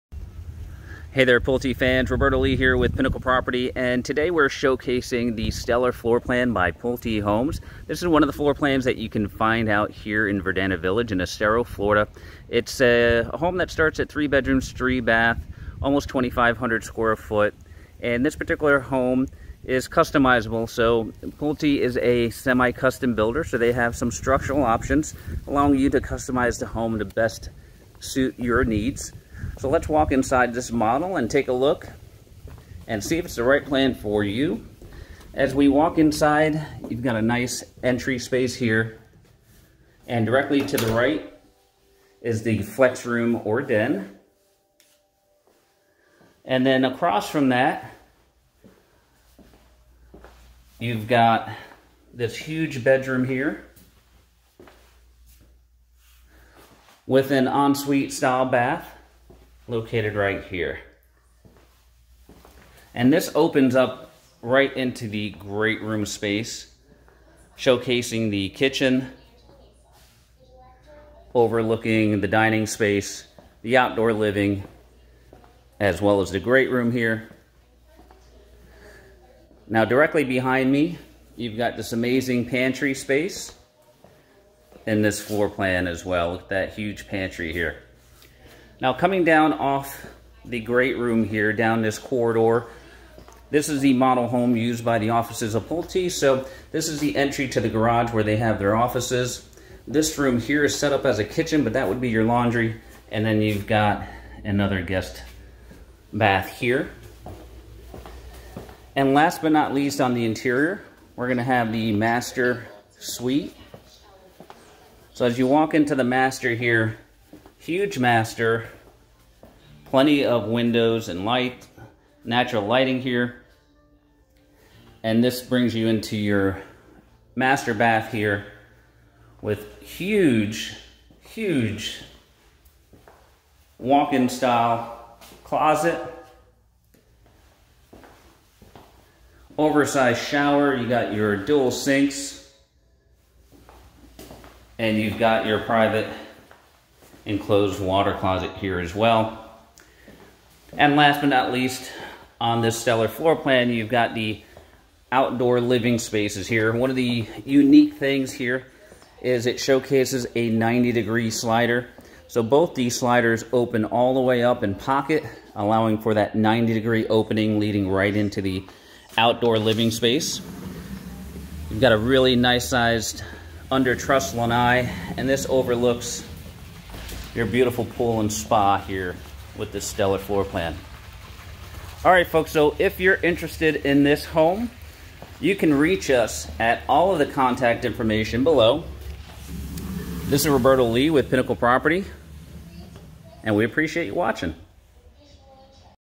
The Stellar Echelon Series Home by Pulte
Visit SouthwestFloridaProperties.com to view available homes!

Able to accommodate both entertaining and relaxed everyday living, the Stellar truly represents an achievement. The kitchen opens to the gathering room, dining area, and covered lanai, to suit both formal and informal socializing. And private spaces like the flex room or Owner’s Suite (with huge walk-in closet) let family members still find “me time.” In the garage, an extra bay can hold a golf cart.